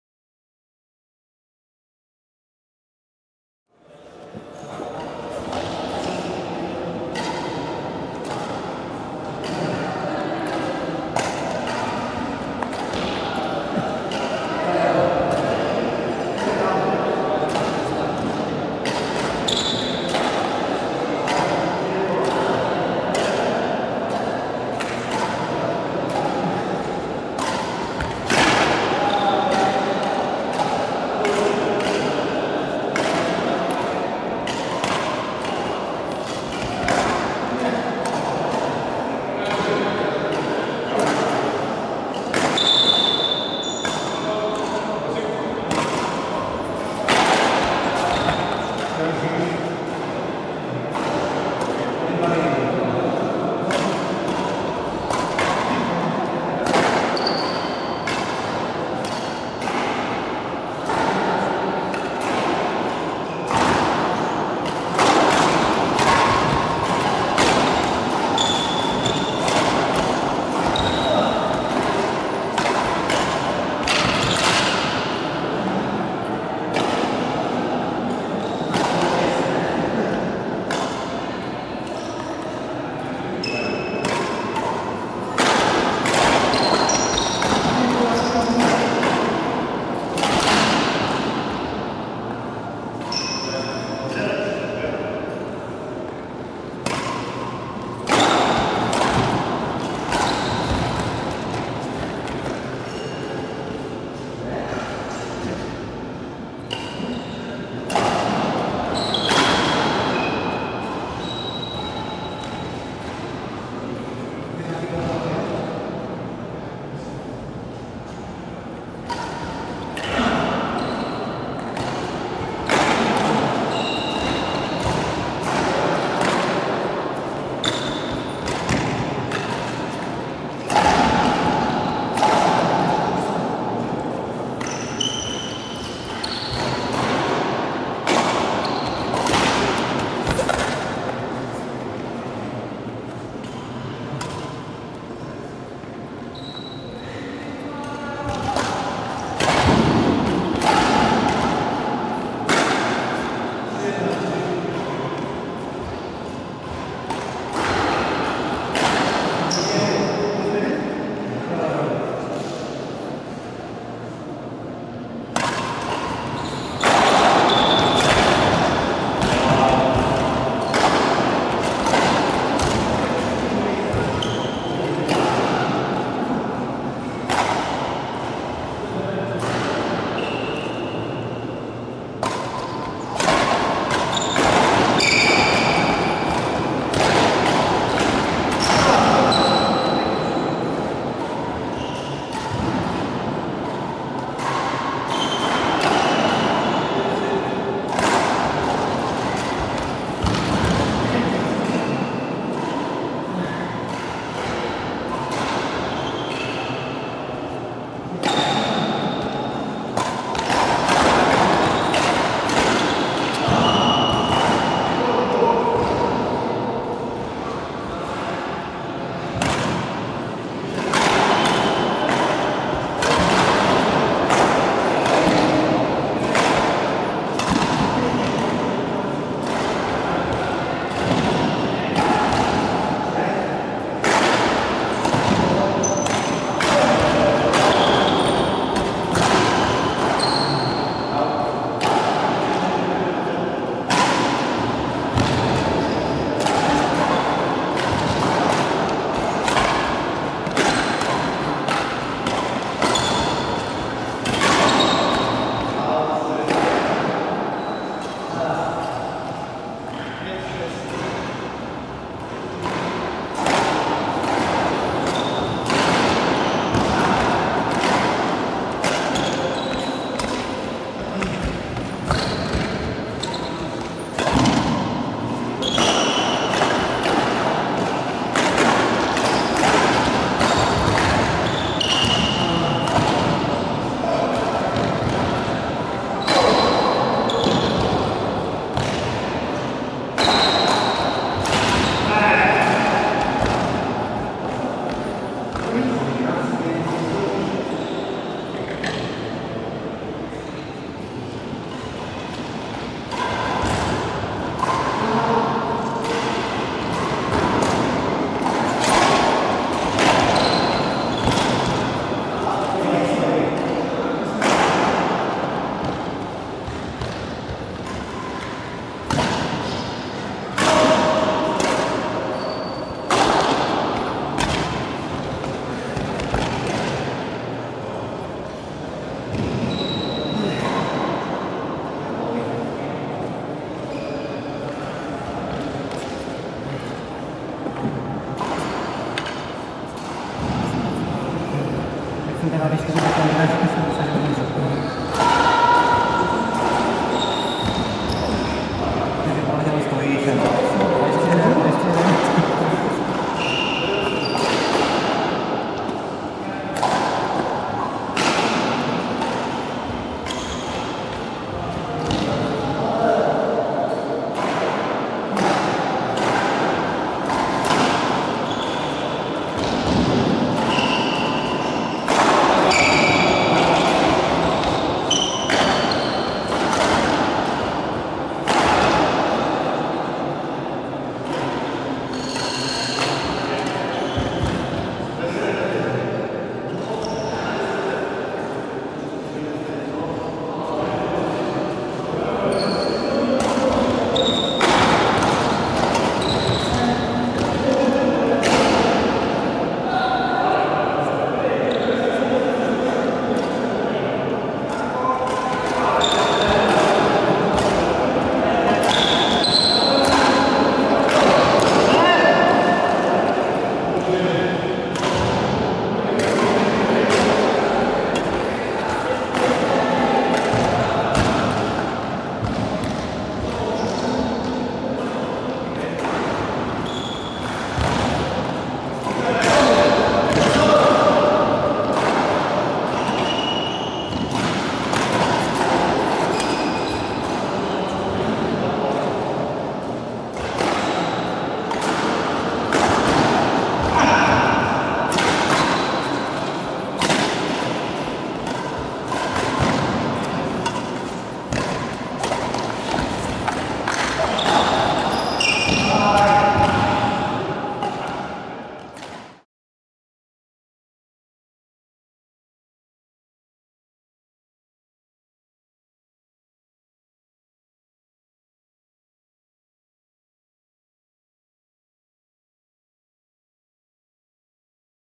2018-04 Badminton - Záznam utkání 9. Ostravský kanár SINGLY B - Roman Fousek vs. Tomáš Karas
Nedohraný zápas z důvodu vyvrtnutého kotníku mezi Romanem Fouskem a Tomášem Karasem z posledního turnaje v této sezóně Ostravského kanára.